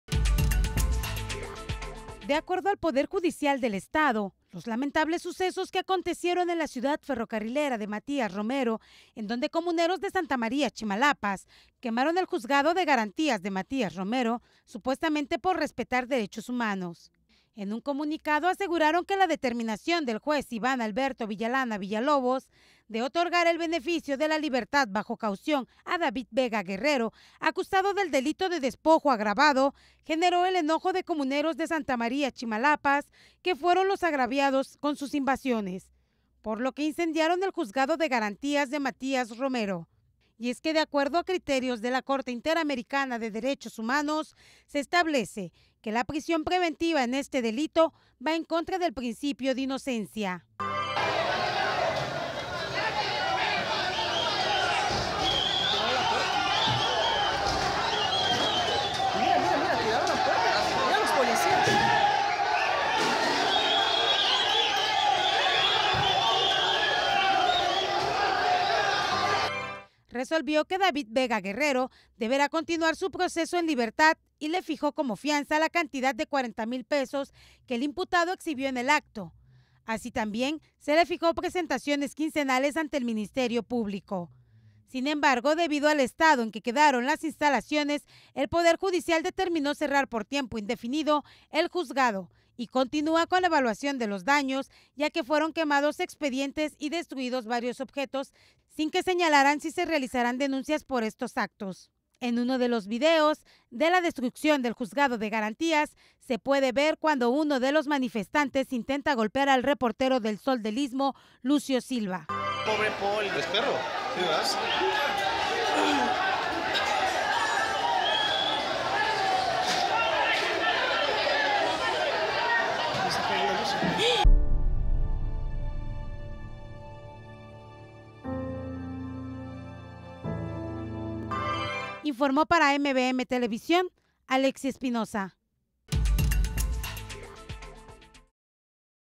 Comuneros de María Chimalapas queman juzgado en Matías Romero.
De acuerdo al Poder Judicial del Estado, los lamentables sucesos que acontecieron en la ciudad ferrocarrilera de Matías Romero, en donde comuneros de Santa María Chimalapas, quemaron juzgado de Matías Romero, supuestamente por respetar derechos humanos.